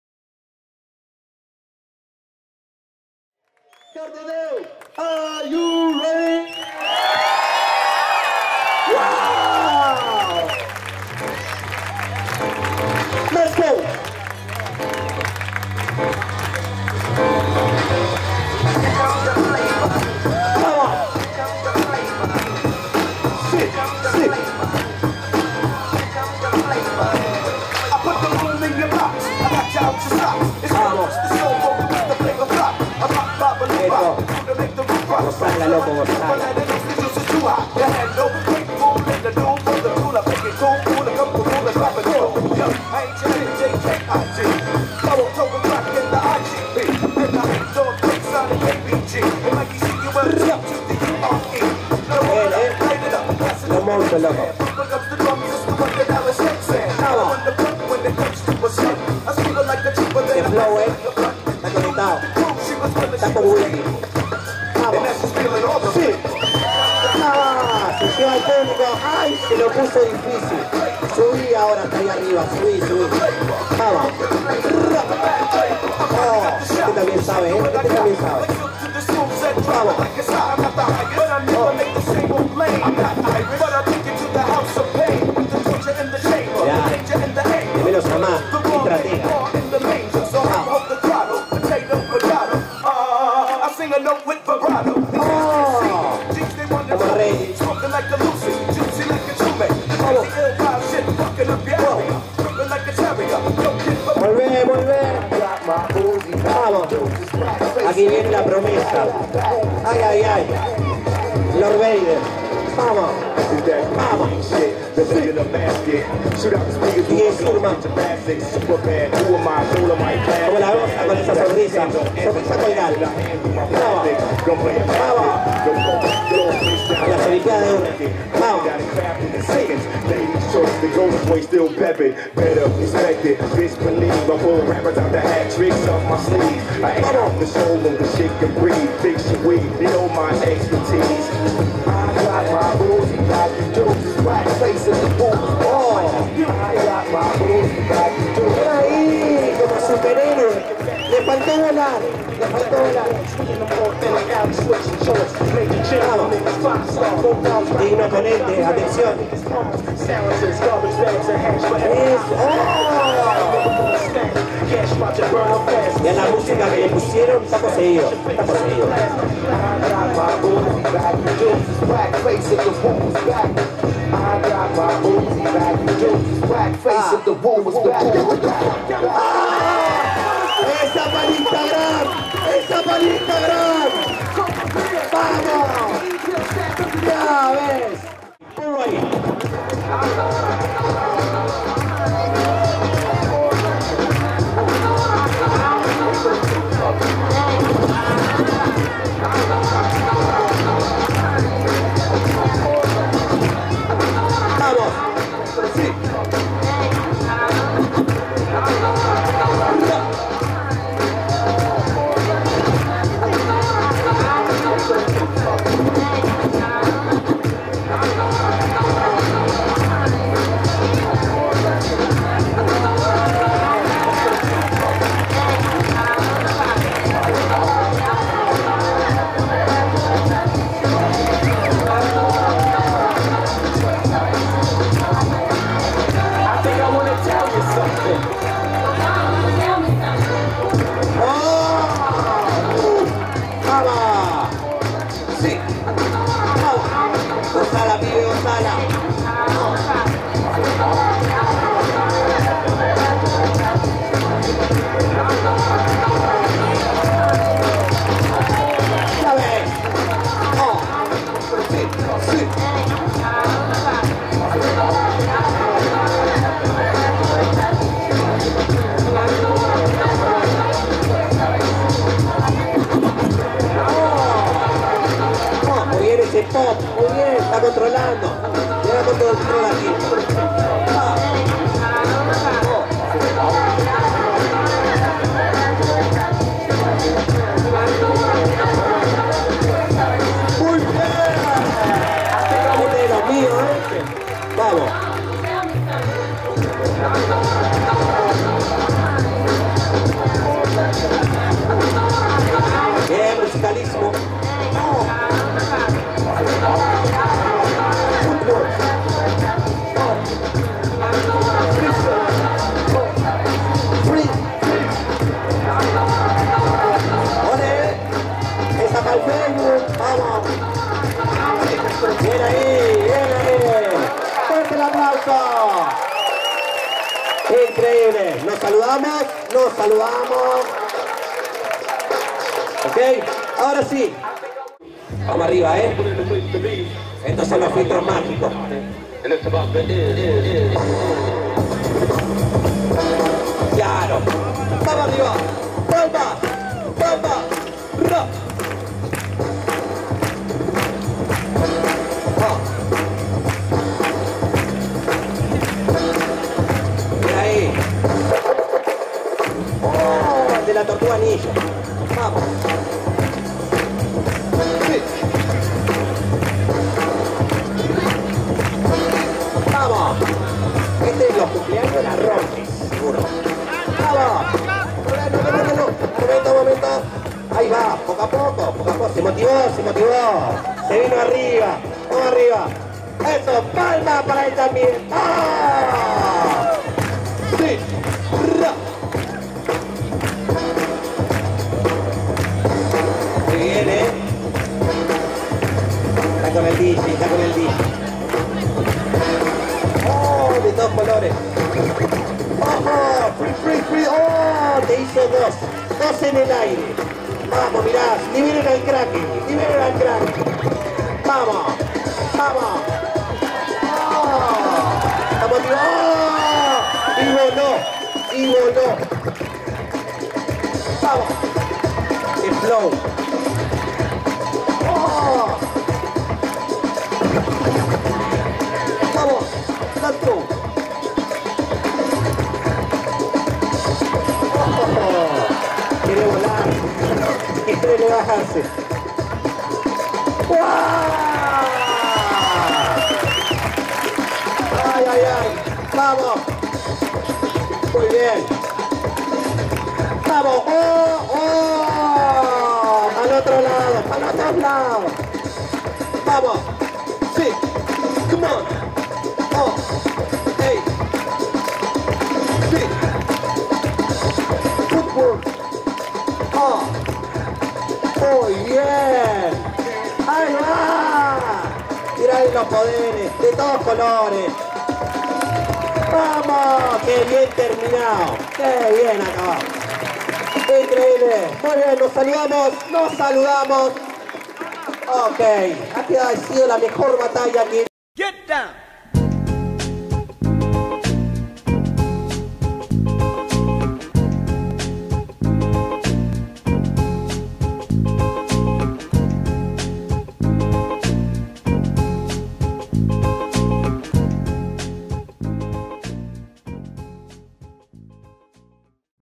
Qualifier Kids EUROSTYLE FORCE 2023 | BREAK STORE
Qualifier about Kids battle 2 vs. 2 on EuroStyle Force 2023, held at 1th October of 2023 in Cardedeu (Barcelona).

- Organizer: Natural Beat
- Host Gumi
- Judges: Natural / Brex / Kadoer
- Dj Surmah